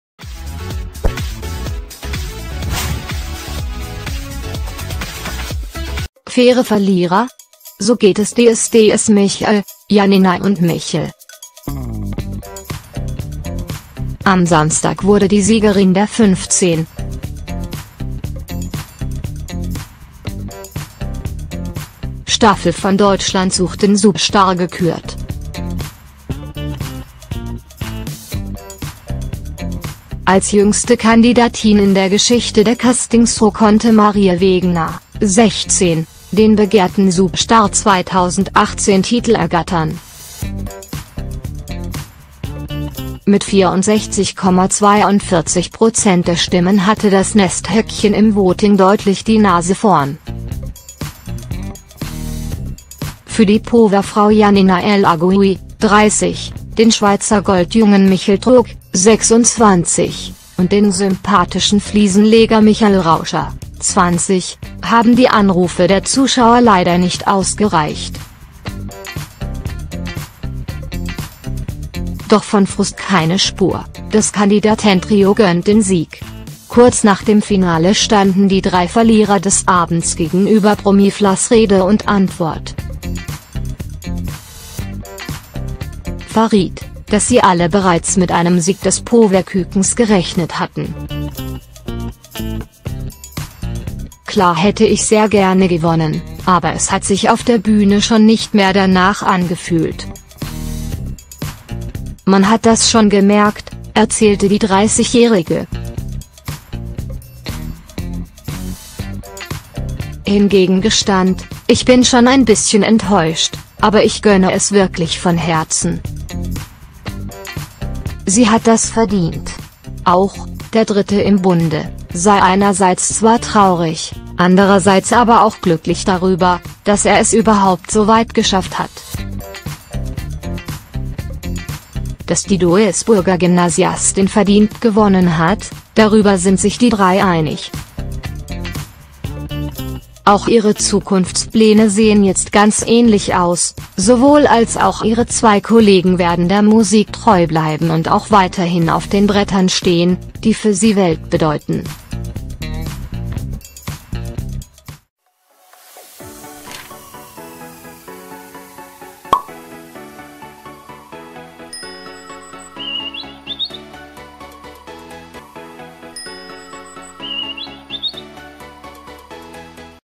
Faire Verlierer? So geht es DSDS-Michael, Janina und Michel
Am Samstag wurde die Siegerin der 15. Staffel von Deutschland sucht den Superstar gekürt. Als jüngste Kandidatin in der Geschichte der Castingshow konnte Marie Wegener (16) den begehrten "Superstar 2018"-Titel ergattern. Mit 64,42 Prozent der Stimmen hatte das Nesthäkchen im Voting deutlich die Nase vorn. Für die Powerfrau Janina El Arguioui (30), den Schweizer Goldjungen Michel Truog (26) und den sympathischen Fliesenleger Michael Rauscher (20) haben die Anrufe der Zuschauer leider nicht ausger